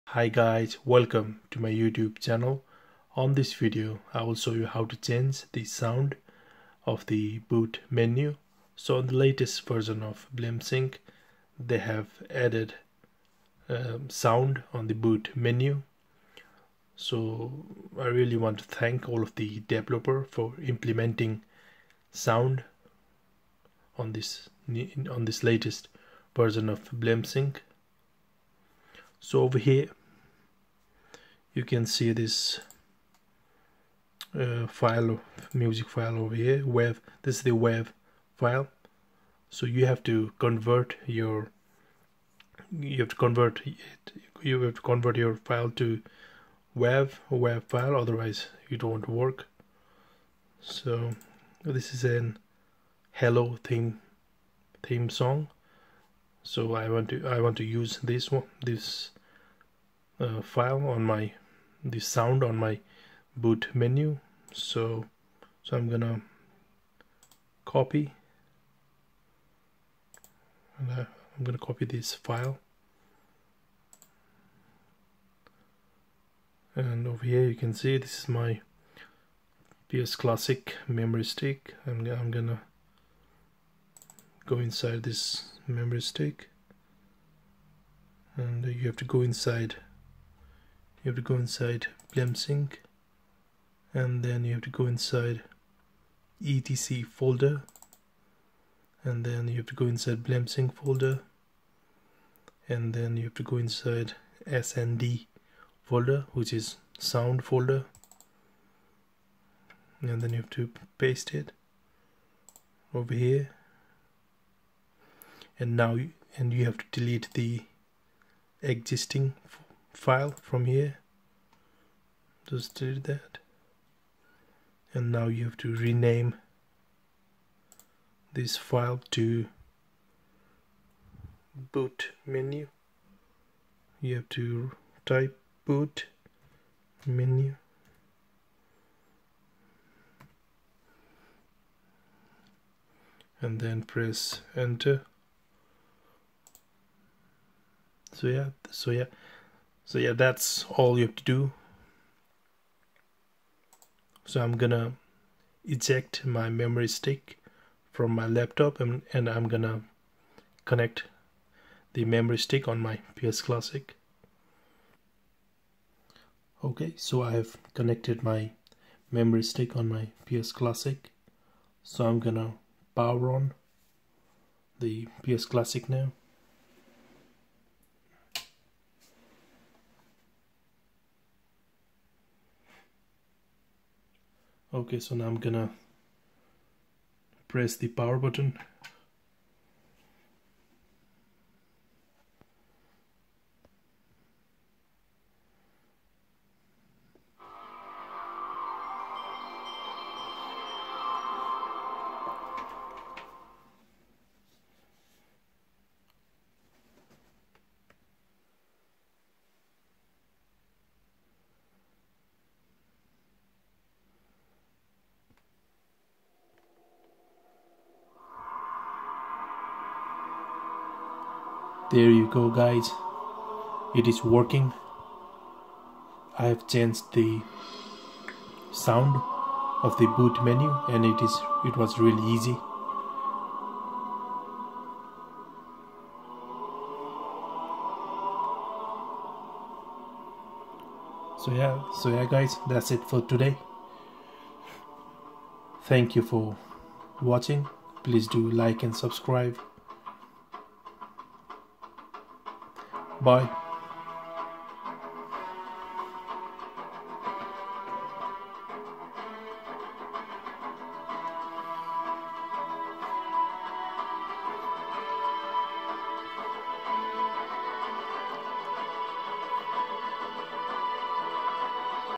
How to change the Sound/Music of the boot menu on Bleemsync 1.0 || ps classic hack || RetroArch
On this video i will show you how to change the sound of the boot menu.